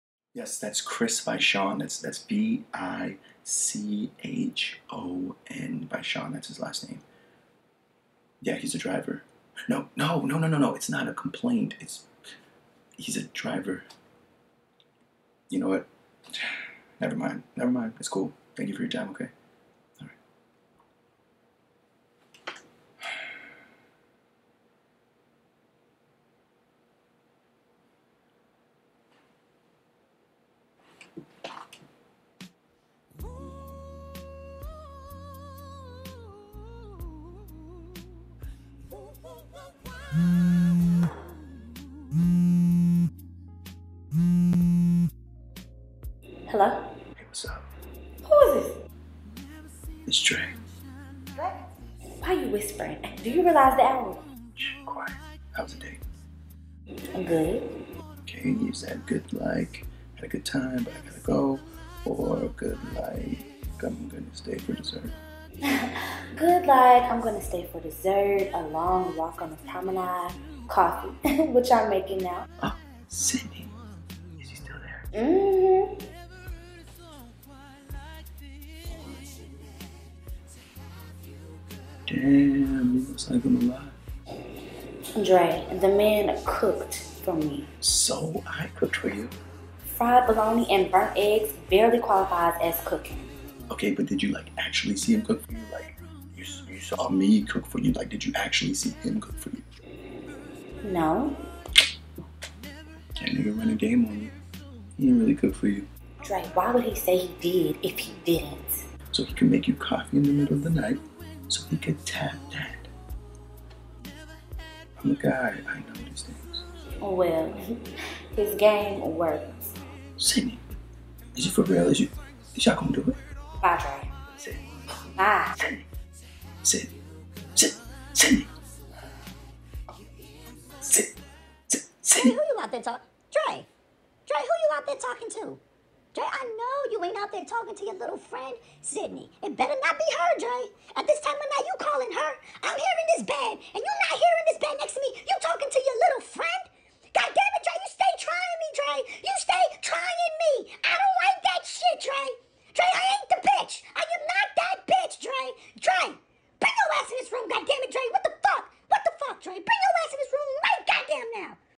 #82 Brown Sugar - Phone Call * The Male Monologue * Borge Etienne
Classic movie with one of my favorite artists (Mos Def). This classic starred Taye Diggs and Sanaa Lathan as 2 close friends who clearly had a thing for each other but because of circumstances, they went different routes in their love lives. In the end of course, they find their way back to each other. The great thing about this movie is the music was their centrifuge and we all know, "you can't escape the music". My friend Kelsey Brooke was able to grace me with her collab in this scene with me as of course we added our own spin to it. Watch the whole thing and "wait for it" at the end!

Director Rick Famuyiwa
ScreenWriter Michael Elliot